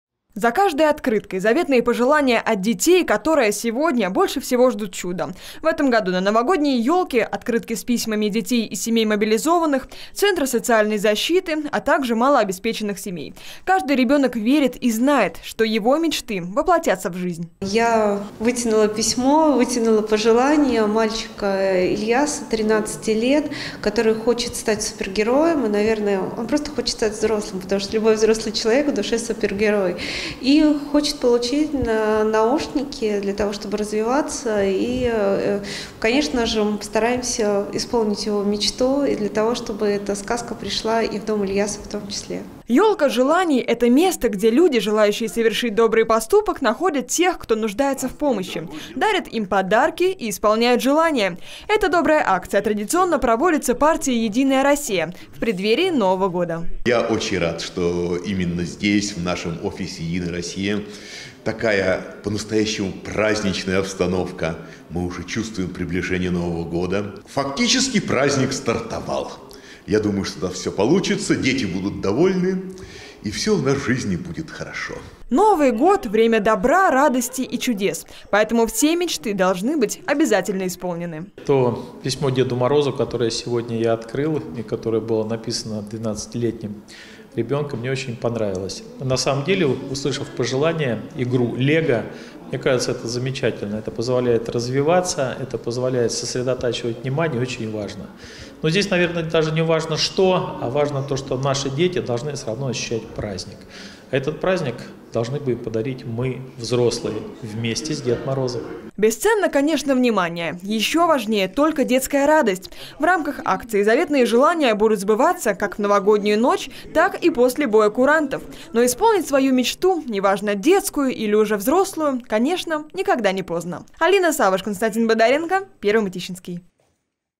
«Ёлка желаний»: в Мытищах стартовала акция «Единой России»
До Нового года остаётся чуть больше недели. Это время приятных хлопот, выбора подарков и добрых дел. Одно из них – «Ёлка желаний». Эта добрая акция ежегодно проводится партией «Единая Россия». Чьи мечты исполнятся в этом году? (12+)